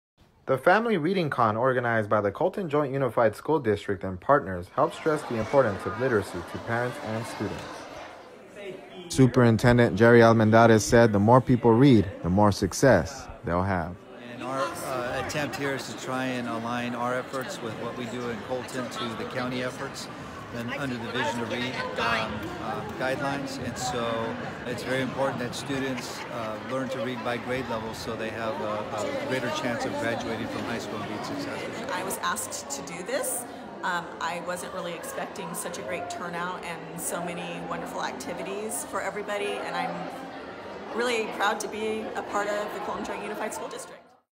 CJUSD Family Reading Con
The first ever Family Reading Con held by The Colton Joint Unified School District and partners over the weekend stressed the importance of literacy to parents and students.